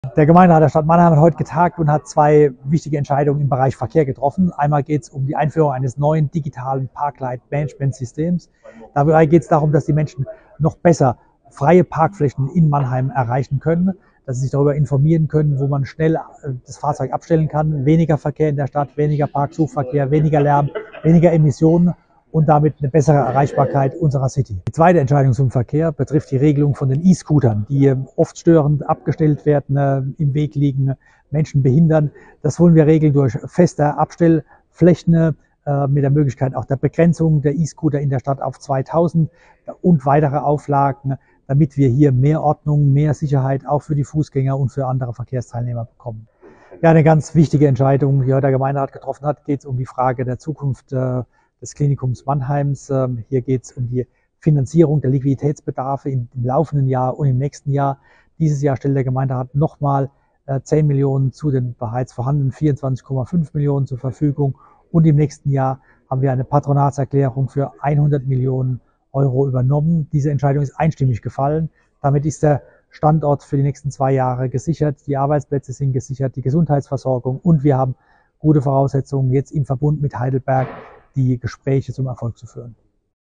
Bericht Gemeinderat 14. Mai 2024 OB Specht
Gestern ist der Gemeinderat Mannheim zu seiner Mai-Sitzung zusammengekommen. Oberbürgermeister @christian.specht.ma fasst einige Ergebnisse kurz zusammen.

Die nächste Gemeinderatssitzung ist am Dienstag, 13. Juni. Ihr seid herzlich eingeladen, sie live im Stadthaus N1 oder im Stream auf mannheim-videos.de zu verfolgen.